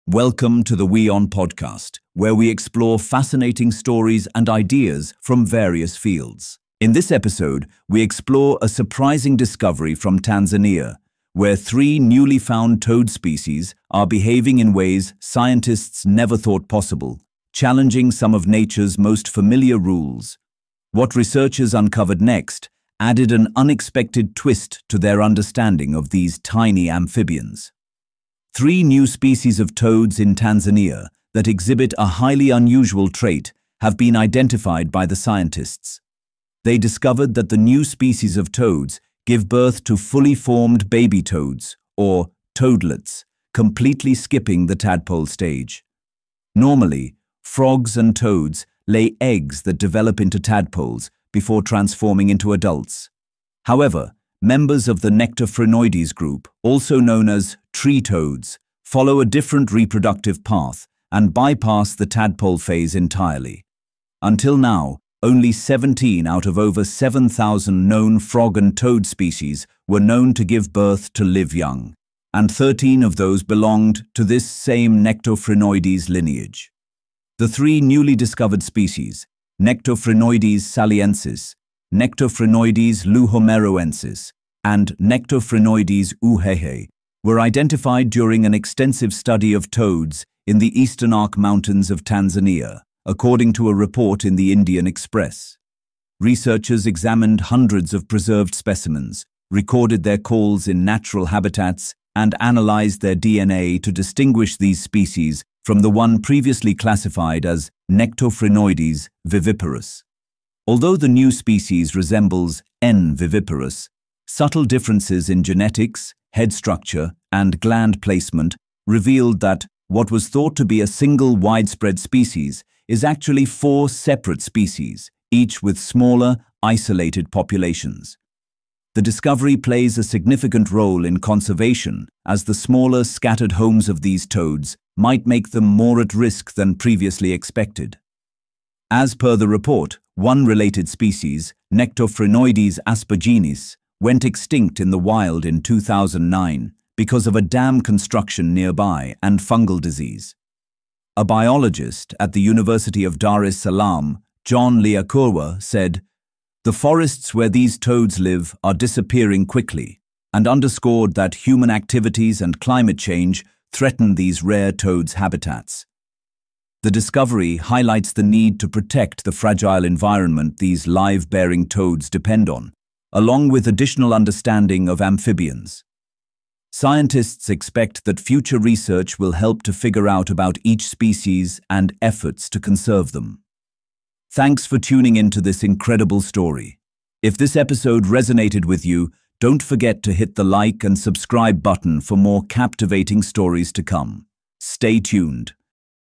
These 3 Newly Found Tanzanian Toads Break Nature’s Rules | WION Podcast
Scientists in Tanzania have identified three remarkable new toad species that overturn what we thought we knew about amphibian reproduction. Unlike most frogs and toads that begin life as tadpoles, these species give birth to fully formed baby toads, skipping the larval stage entirely. Detailed research revealed that what was believed to be one species is actually four, each with limited and vulnerable habitats in the Eastern Arc Mountains. Their discovery highlights both an evolutionary mystery and an urgent conservation challenge as their forests rapidly disappear. These findings shine a spotlight on how rare and fragile live-bearing amphibians truly are.